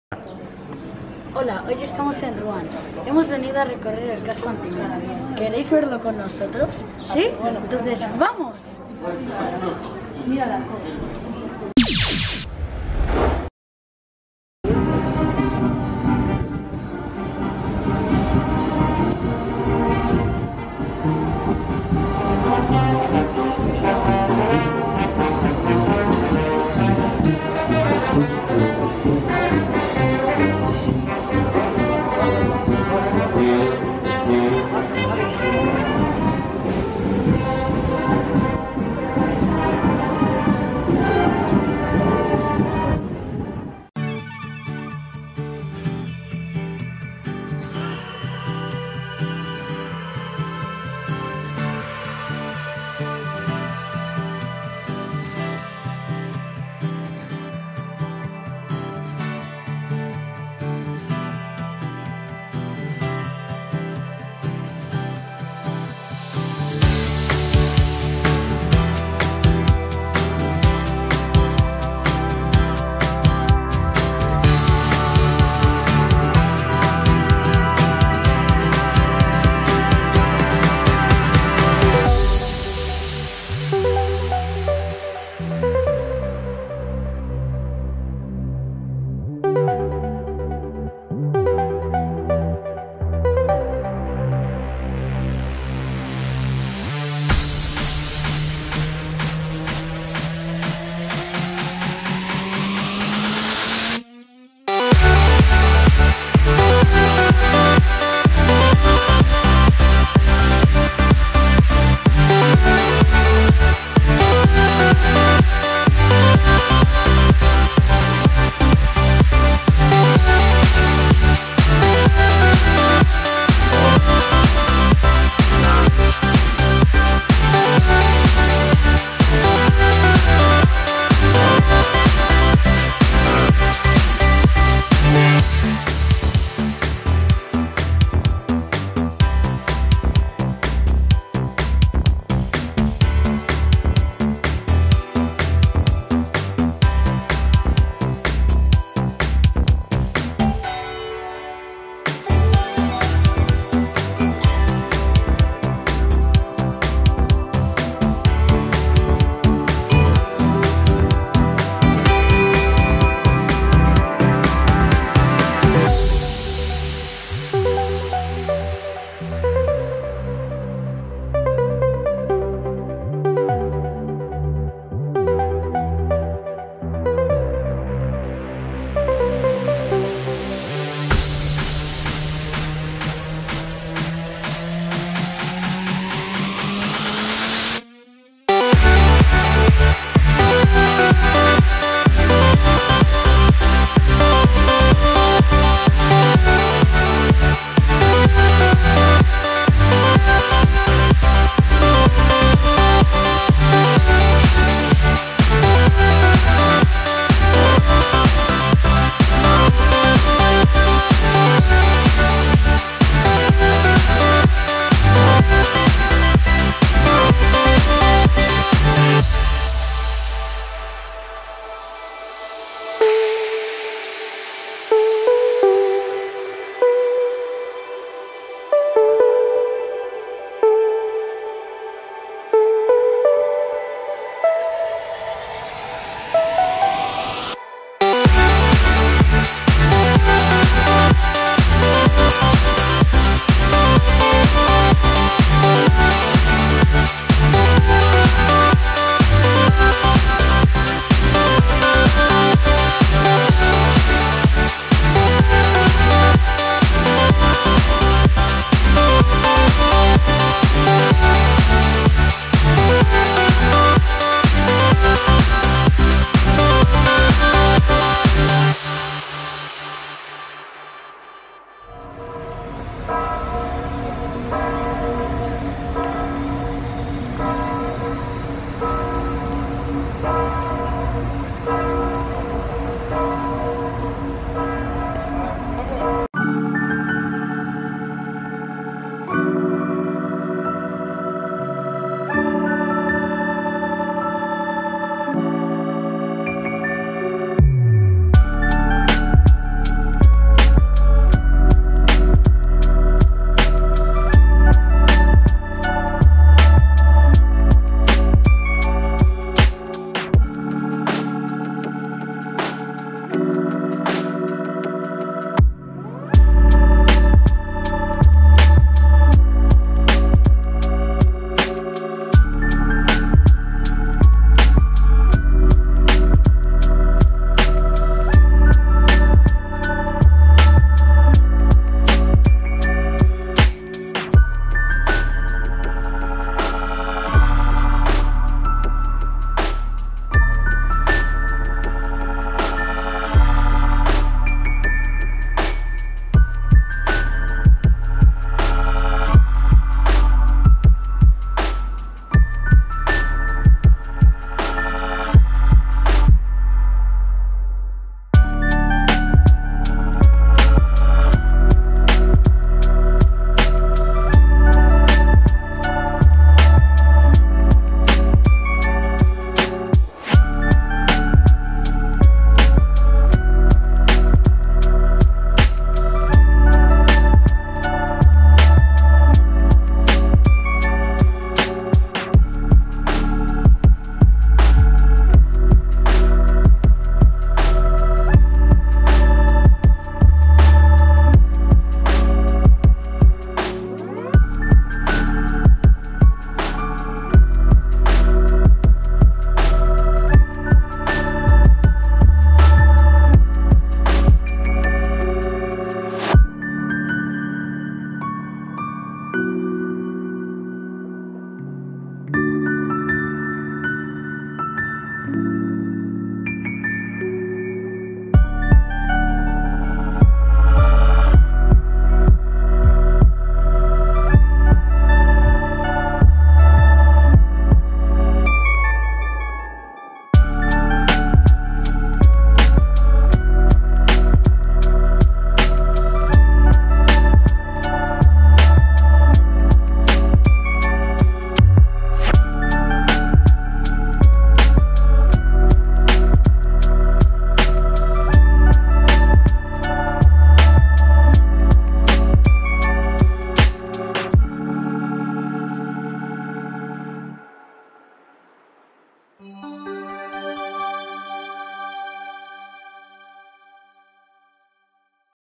Rouen (Ruan), capital de la región de Normandía | Francia #15
Rouen (Ruan), capital de la región de Normandía, en el norte de Francia, es una ciudad portuaria a orillas del río Sena. Fue una ciudad importante en la época romana y en la Edad Media, lo que se aprecia en sus iglesias góticas, como Saint-Maclou y Saint-Ouen, y su centro peatonal adoquinado con casas medievales de entramado de madera. Las torres de la catedral de Notre-Dame, que aparece en numerosos cuadros del pintor impresionista Claude Monet, se alzan sobre la silueta urbana de esta ciudad.
Descubre nuestro canal de viajes en familia de Youtube.